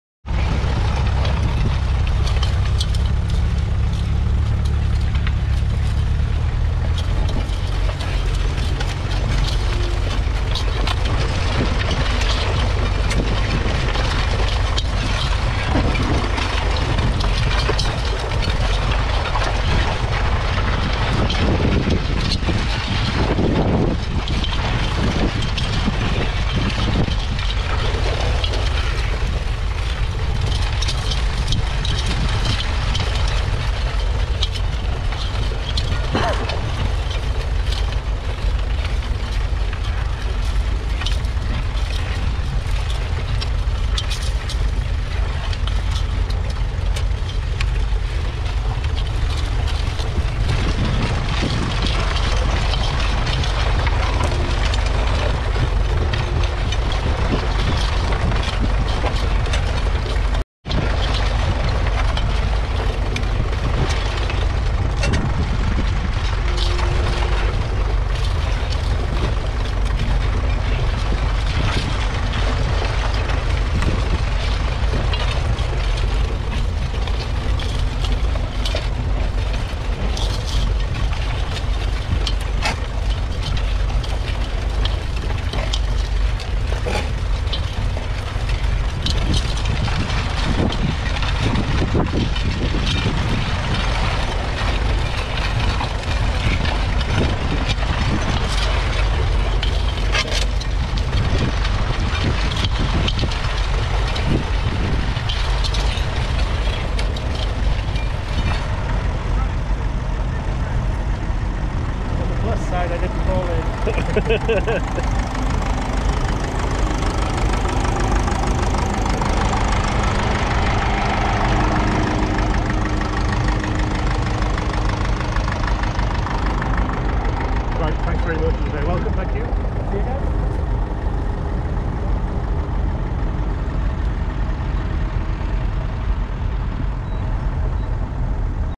1953 Ferguson TEF20 2.1 LItre 4-Cyl Diesel Tractor (28HP) With Ferguson Plough
1953 Ferguson TEF-20 2.1 LItre 4-Cyl Diesel Tractor (28HP)
Ferguson 2 Furrow Mounted Plough

Whitchurch YFC Ploughing Match 2022 - Stoke Manor, Market Drayton, Shropshire, England, United Kingdom - Saturday 26th Match 2022